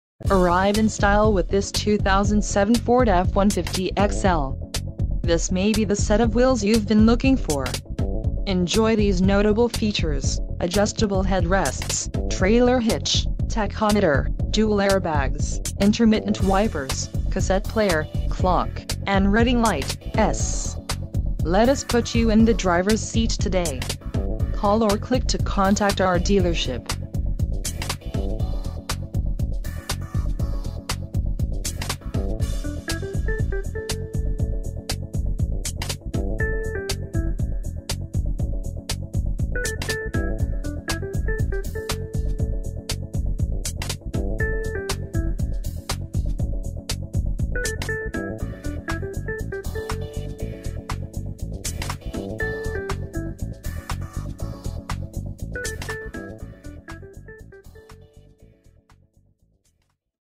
2007 Ford F-150 - Laird Noller Hutchinson - Bad or No Credi
Arrive in style with this 2007 Ford F-150 XL. This may be the set of wheels you've been looking for. Enjoy these notable features: Adjustable Head Rests, Trailer Hitch, Tachometer, Dual Air Bags, Intermittent Wipers, Cassette Player, Clock, and Reading Light(s). Let us put you in the drivers seat today. Call or click to contact our dealership..